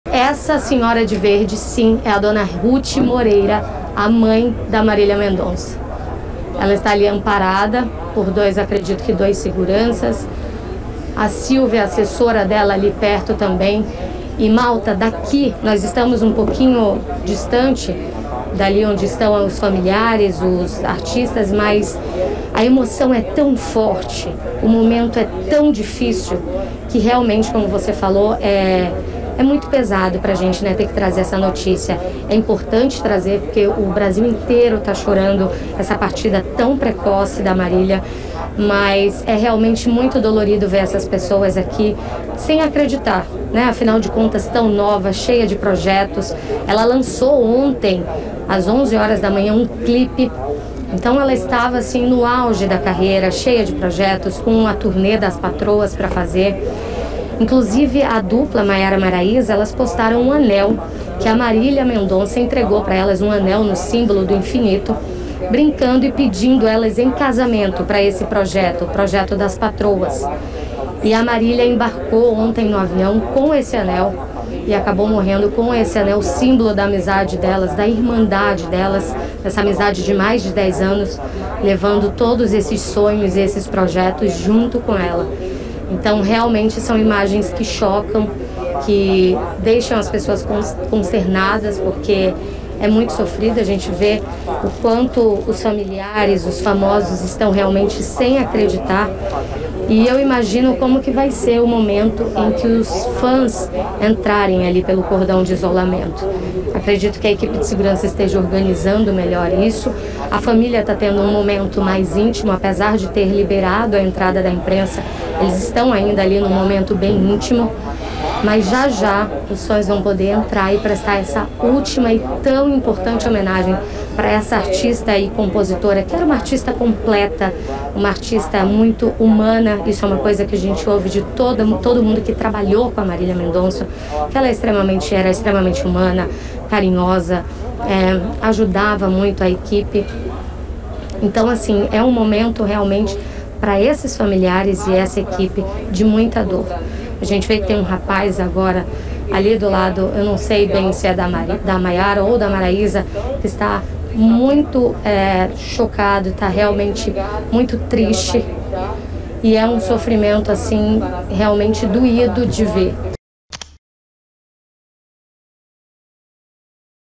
Familiares se despedem de MARÍLIA MENDONÇA: VELÓRIO
No velório da cantora Marília Mendonça, familiares e amigos se despendem após trágico acidente de avião.

A dupla Maiara e Maraisa ainda fez revelação sobre a amizade com Marília. Vejo no vídeo todos os detalhes desse momento difícil para muitos brasileiros.